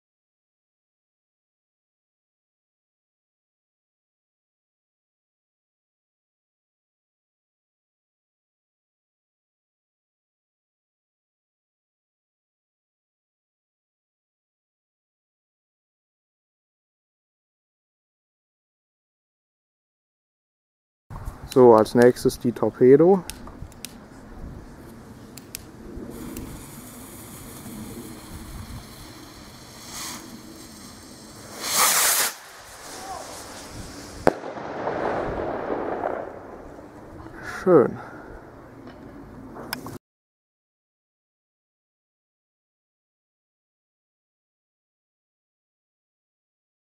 Torpedo Keller
Das war die Torpedo von Keller aus dem Sortiment Nofretete, gefilmt mit der neuen HD Kamera.

Viel Spaß!

Die letzte Rakete des Sets wird morgen hochgeladen! :D

DerFeuerwerkChannel